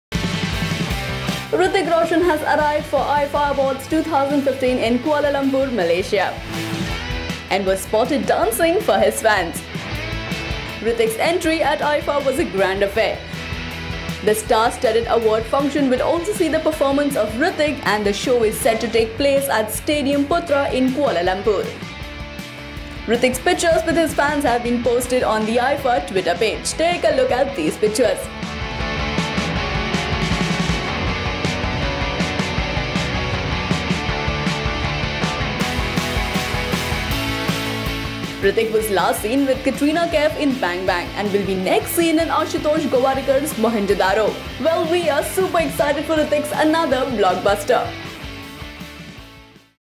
IIFA 2015: Hrithik Roshan's Grand Entry
Hrithik Roshan's entry at the IIFA awards 2015 was a grand one. Click to know more.